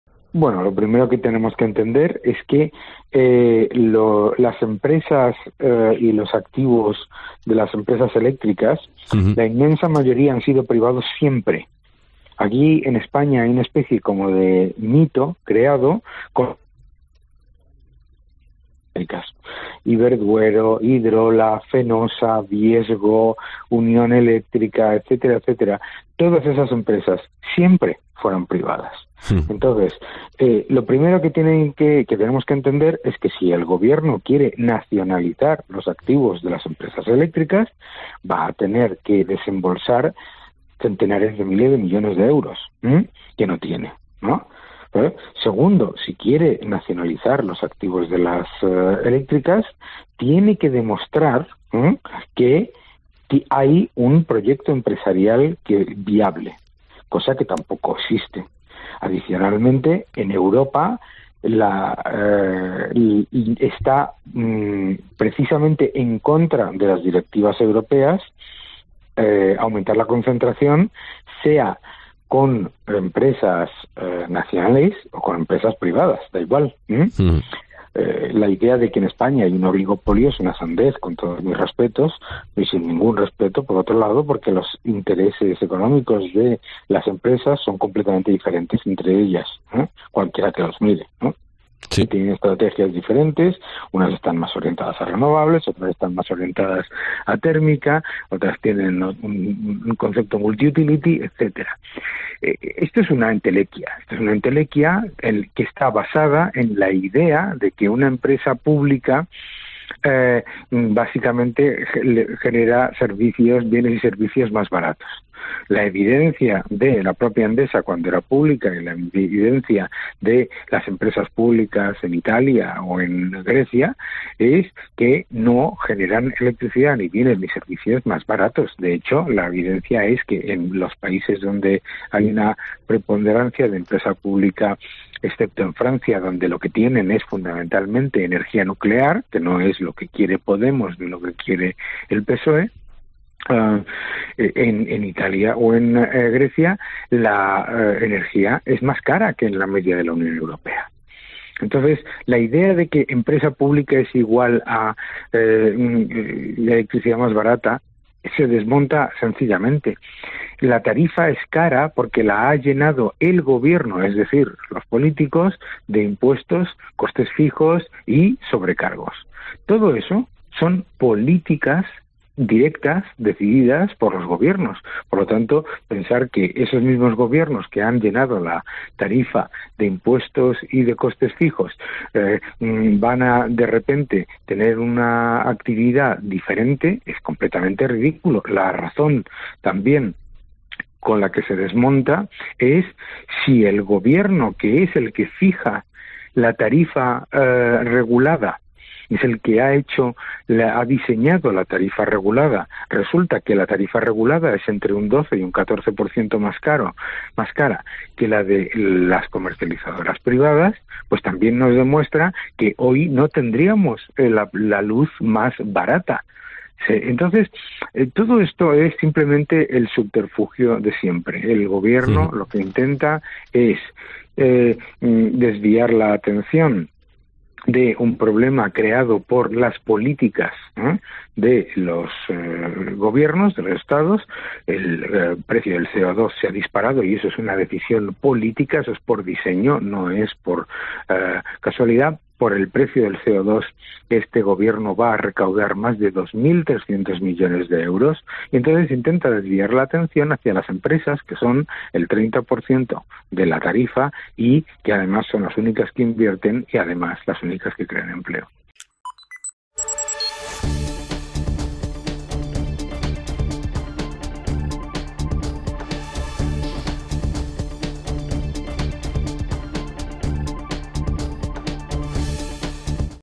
"La tarifa de la luz es cara porque la ha llenado el Gobierno de impuestos y costes fijos"
El economista y escritor Daniel Lacalle Fernández, ha hablado en COPE.es para explicar el fenómeno de la propuesta impulsada por Unidas Podemos de crear una empresa pública para la gestión energética en España. Según Lacalle, lo primero que hay que entender es que todo este tipo de empresas han sido privadas siempre, dado que en España hay un "mito" creado sobre las empresas de energía. Por lo tanto, "si el Gobierno pretende nacionalizar los activos de las empresas eléctricas va a tener que desembolsar centenares de miles de millones de euros, algo que es imposible. Además tiene que demostrar que hay un proyecto empresarial viable, algo que no existe ahora mismo. La idea de que en España hay un oligopolio es una sandez, pues cada empresa tiene unos intereses económicos diferentes" explica.